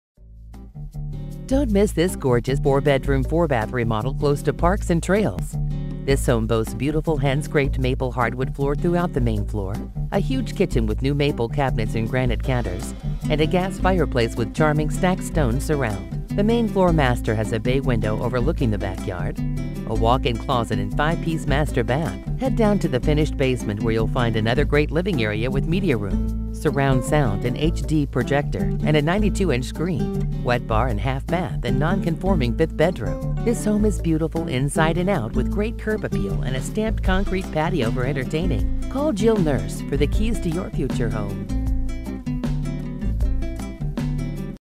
9925 Keenan Street HIGHLANDS RANCH, CO Homes for Sale | coloradohomes.com
9925 Keenan Street, HIGHLANDS RANCH, CO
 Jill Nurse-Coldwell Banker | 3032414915

GORGEOUS REMODEL WITH MAIN FLOOR MASTER, HIGH END CHEFand#x2019;S KITCHEN THAT INCLUDES NEW MAPLE CABINETS, GRANITE COUNTERS, HUGE KITCHEN ISLAND THAT SEATS 6, STAINLESS APPLIANCES INCLUDING A 5 BURNER GAS STOVE IN A SEPARATE COOKand#x2019;S ISLAND. BEAUTIFUL HAND SCRAPED MAPLE HARDWOOD FLOORS THROUGHOUT THE MAIN FLOOR, HIGH CEILINGS AND GAS FIREPLACE WITH CHARMING STACKED STONE SURROUND. THE MAIN FLOOR MASTER HAS A BAY WINDOW OVERLOOKING THE BACKYARD, WALKIN CLOSET and 5 PIECE MASTER BATH. THE FINISHED BASEMENT HAS ANOTHER GREAT LIVING AREA WITH MEDIA ROOM (HD PROJECTOR WITH 92&quot; SCREEN AND SURROUND SOUND), WET BAR, 1/2 BATH AND NON-CONFORMING 5TH BEDROOM OR OFFICE. THIS HOME IS BEAUTIFUL INSIDE AND OUT WITH GREAT CURB APPEAL, PROFESSIONAL LANDSCAPING, AND STAMPED CONCRETE PATIO. TOP RATED DOUGLAS COUNTY SCHOOLS and A SHORT WALK TO REDSTONE ELEMENTARY. HOA